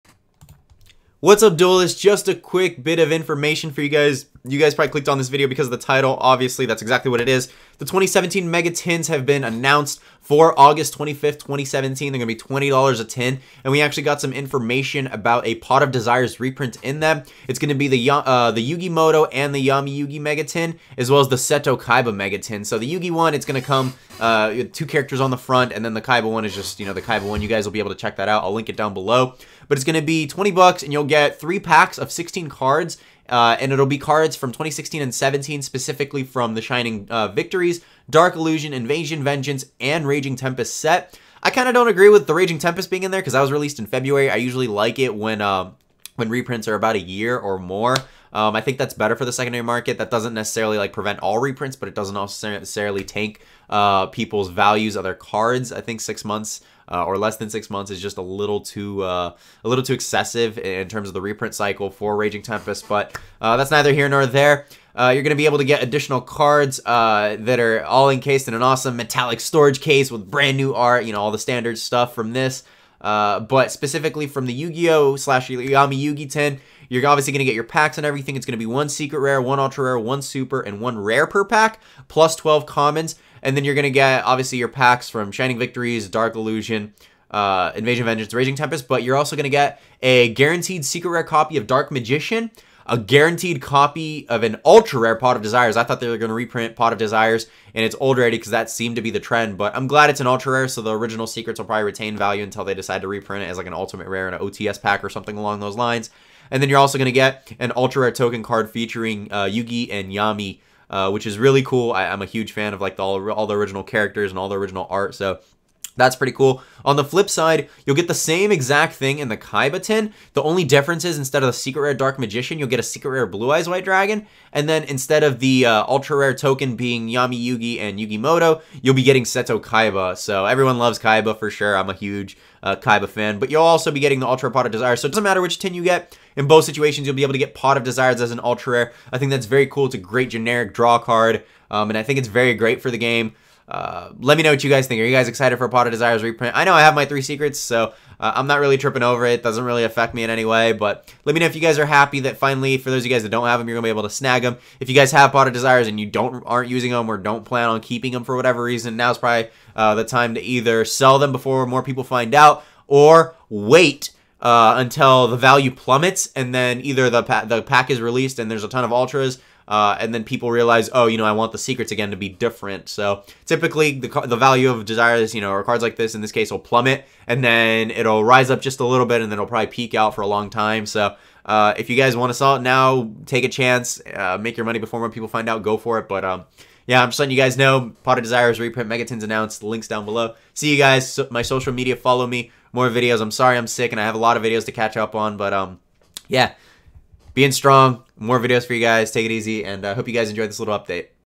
YUGIOH POT OF DESIRES MEGA-TINS REPRINT AS AN ULTRA RARE!!!
XBOX LIVE GAMERTAG: intch95
DUELING NETWORK: intch95

"Limits, like fears, are often just an illusion"